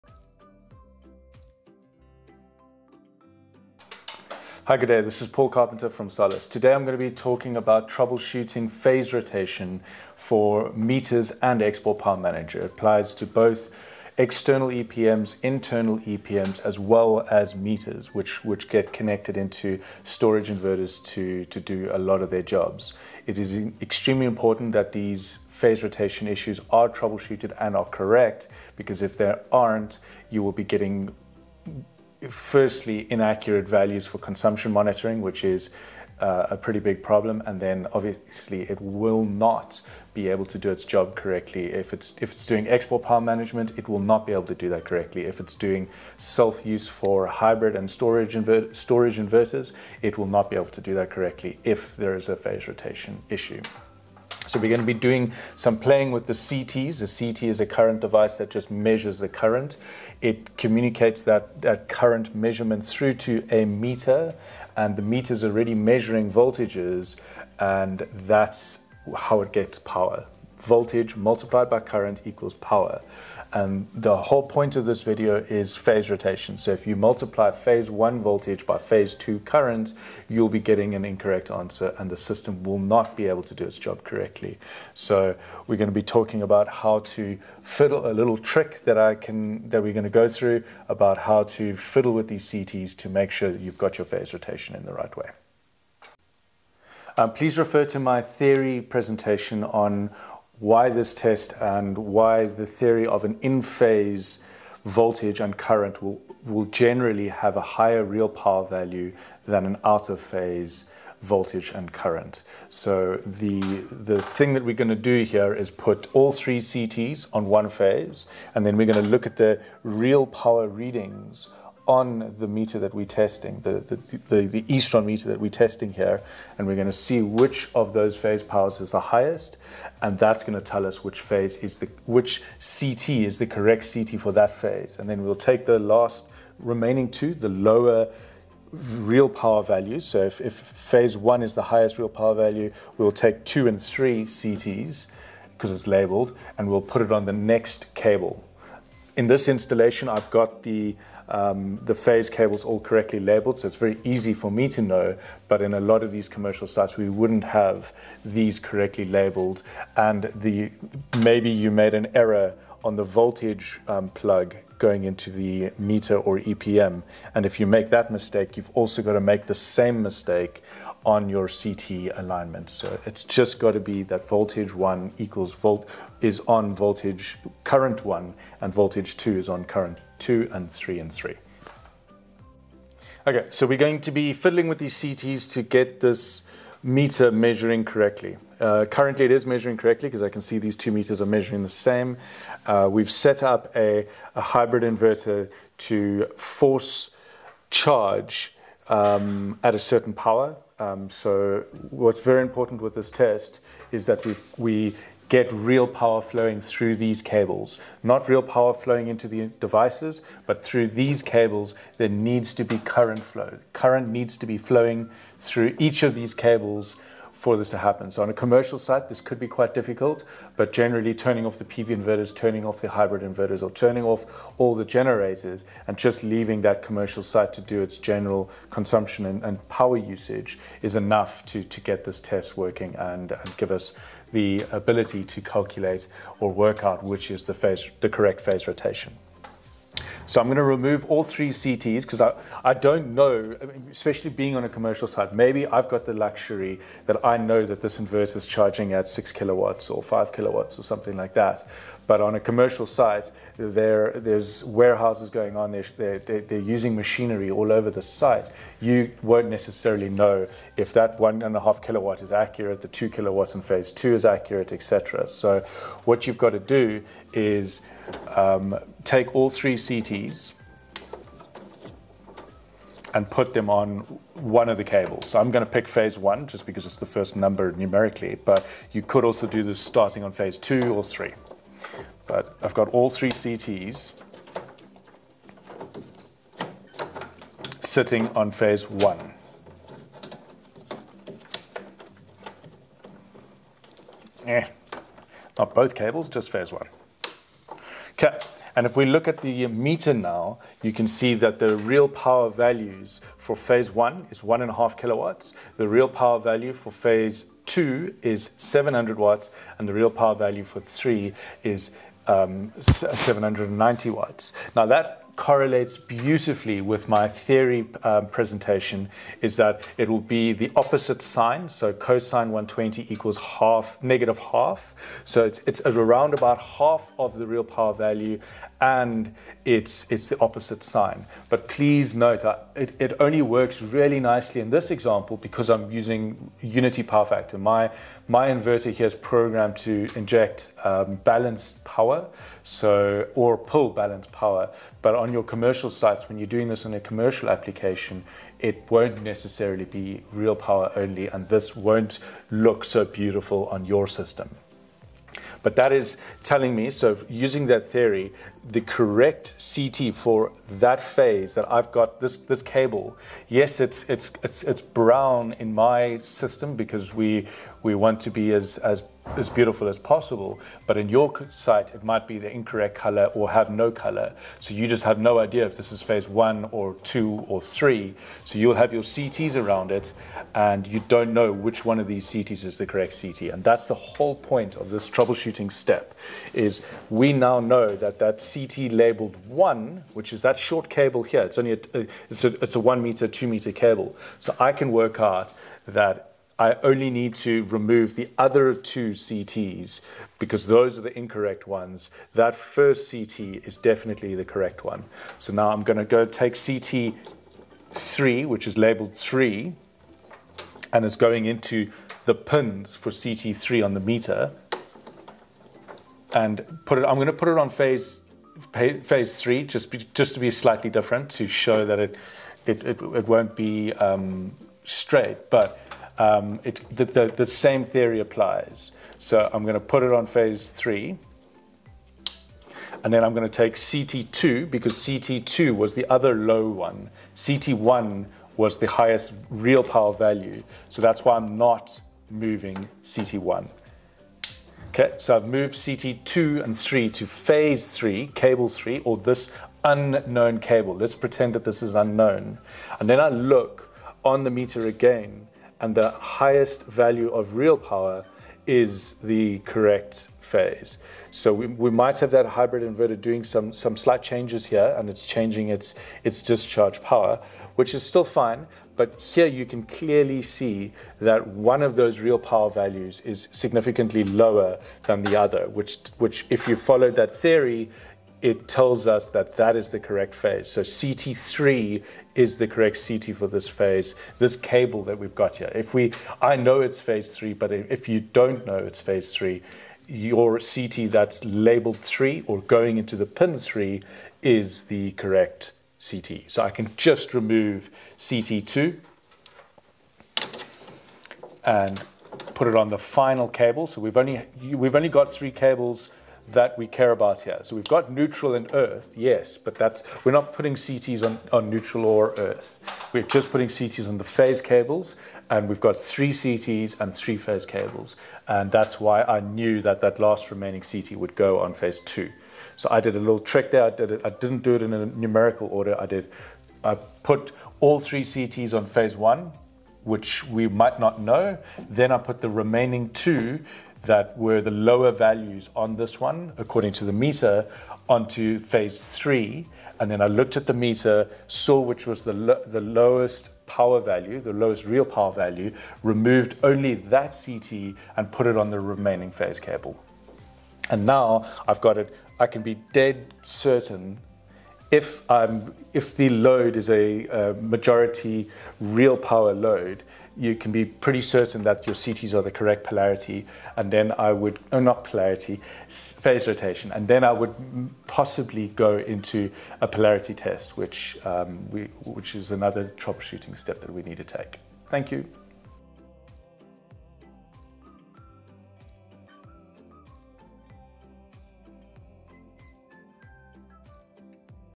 Solis Meter Installation - Phase Rotation
In this video our Technical Manager for Europe explains phase rotation in detail.  He goes through why its important, how to do an isolation test and how to verify its working as intended.